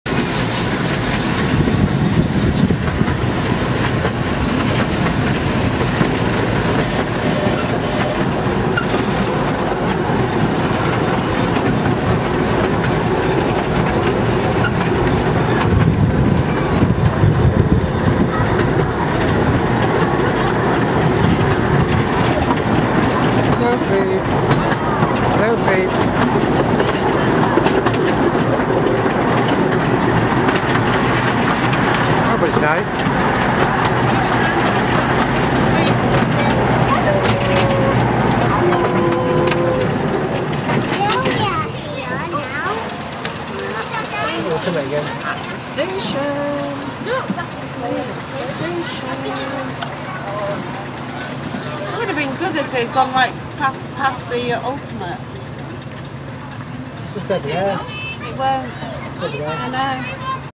Train at lightwater valley. Family day
Train ride at Lightwater Valley. Near ripon.
A nice family day out.